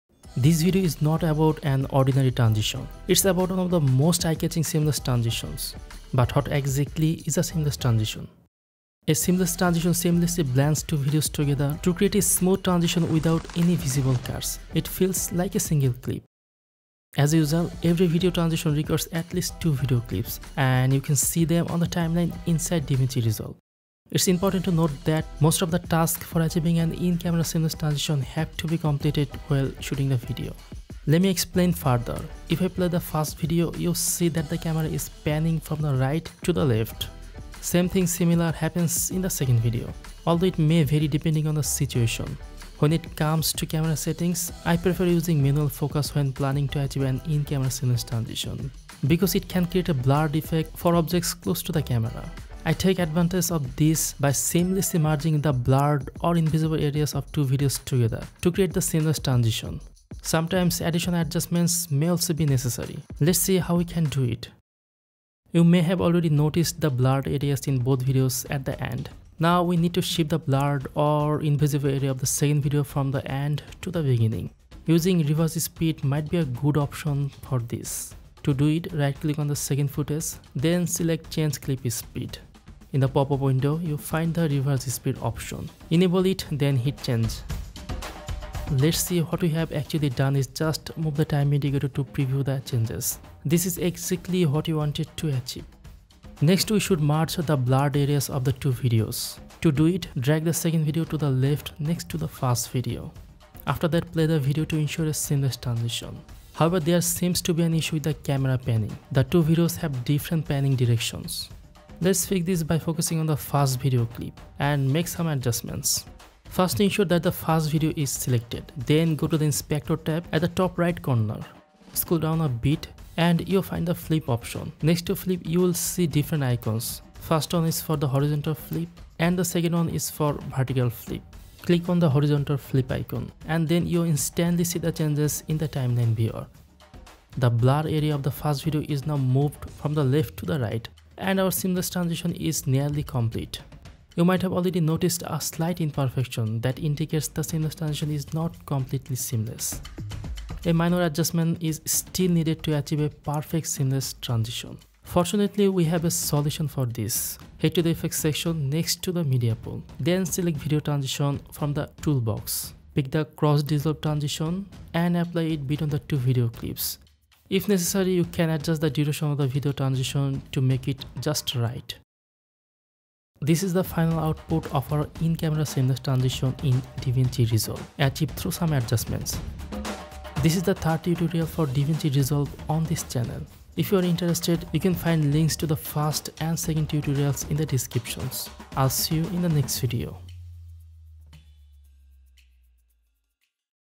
DaVinci Resolve Tutorial: Seamless Transition
This video is not about an ordinary transition; it's about one of the most eye-catching seamless transitions. But what exactly is a seamless transition? A seamless transition seamlessly blends two videos together, creating a smooth transition without any visible cuts; it feels like a single clip. As usual, every video transition requires at least two video clips, and you can see them on the timeline inside DaVinci Resolve. It's important to note that most of the tasks for achieving an in-camera seamless transition have to be completed while shooting the video.

🔥 Save BIG on Editing Tools!